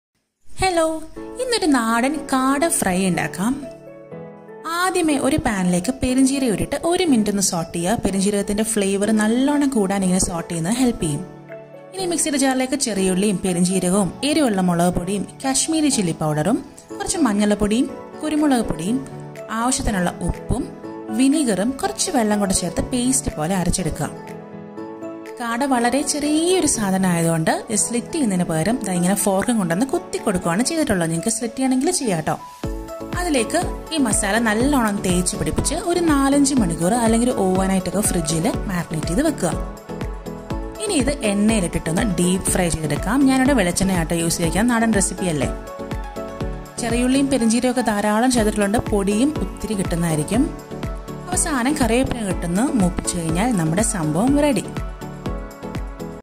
Quail Fry / കാട ഫ്രൈ / Akshayaas Cameo
Ingredients 👇⁣
🍃Quail - 6 to 7 ⁣
🍃Coconut Oil - for deep frying ⁣
🍃Curry Leaves - 2 to 3 strings ⁣